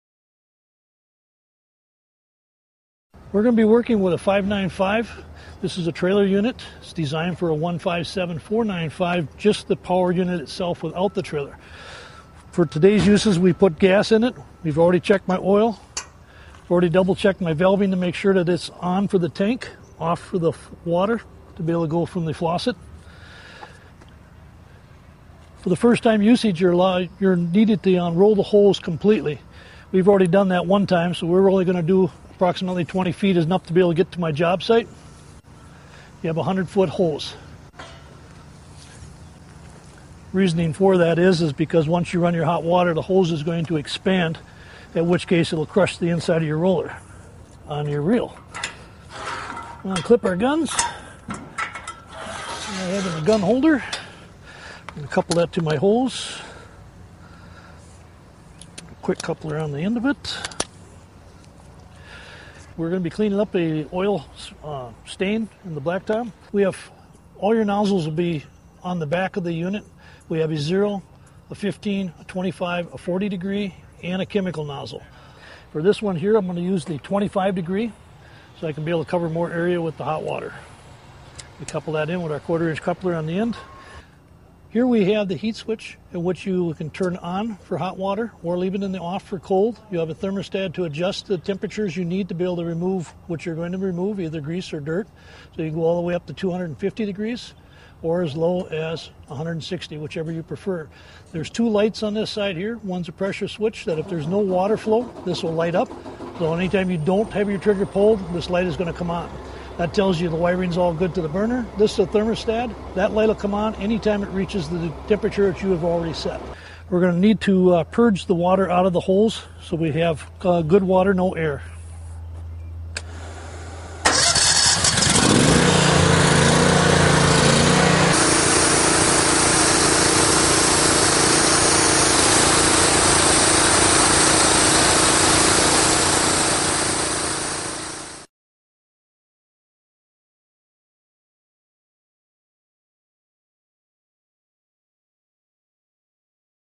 NorthStar Hot Water Pressure Washer - Honda Engine, 4 GPM @ 4000 PSI, Trailer Mounted
Trailer-mounted unit with self-contained 200 gallon water tank delivers hot water washing anywhere! Perfect for remote job sites with no electricity or running water. Honda GX630 V-twin engine powers CAT pump to deliver hot water at 4000 PSI for ...